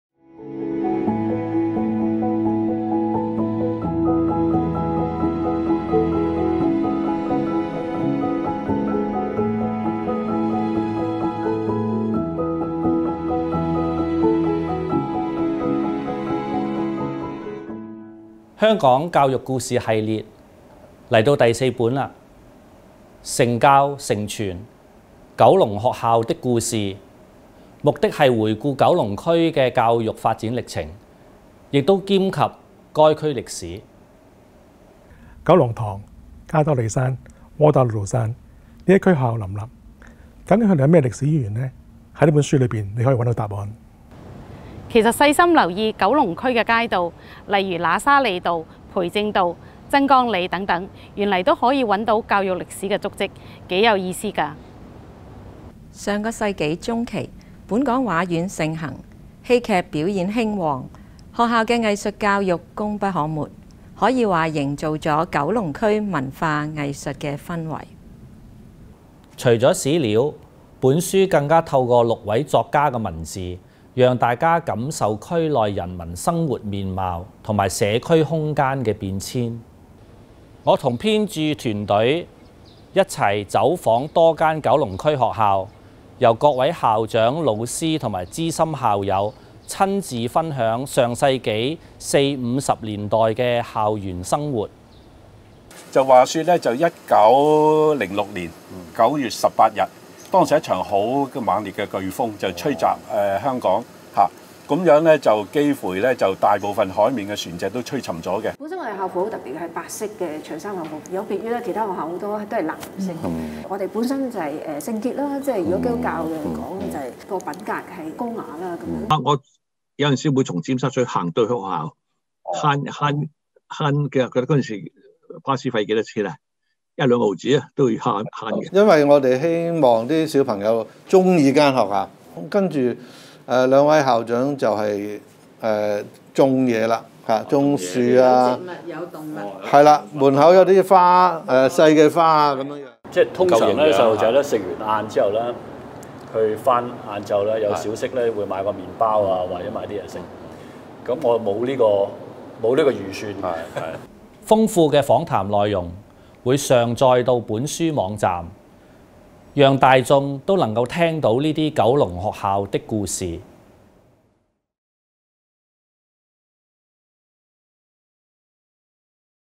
《承教‧ 城傳：九龍學校的故事》宣傳片3
《香港教育故事系列》第四本:《承教 ‧ 城傳：九龍學校的故事》旨在回顧九龍區的教育發展歷程，亦兼及該區歷史。

編者更帶領編著團隊走訪多間九龍區學校，由各位校長、老師和資深校友親自分享上世紀四五十年代的校園生活。

豐富的訪談內容，會上載至本書網站，讓大眾都能聽到這些「九龍學校的故事」。